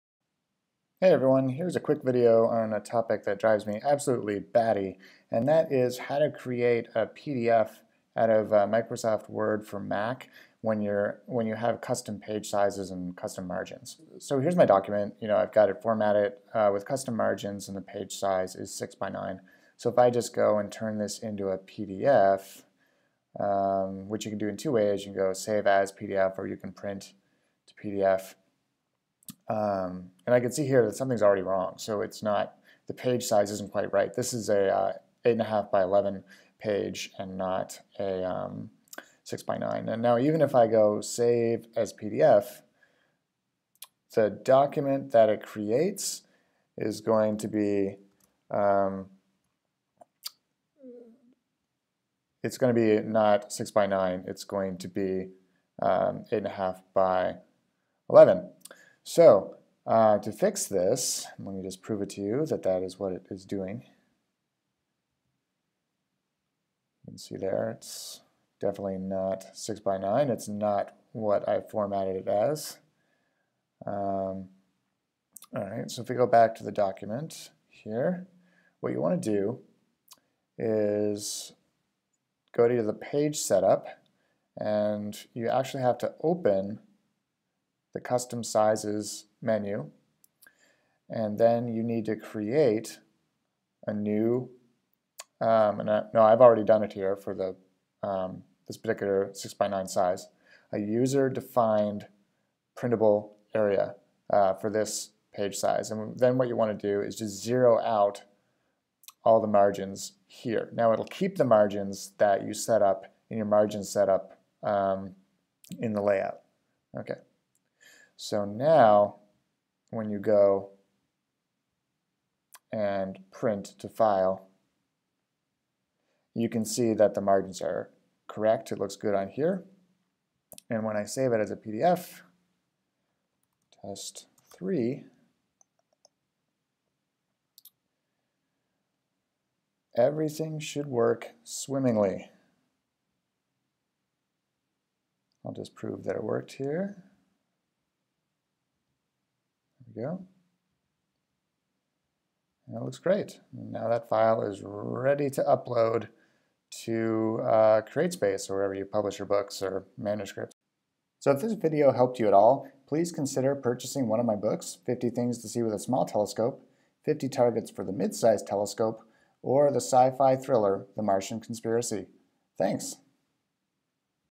Saving as PDF with Custom Margins and Page Sizes in Word for Mac
Having trouble creating a PDF in Word for Mac? Margins and Page sizes causing the issue? Here's the fix.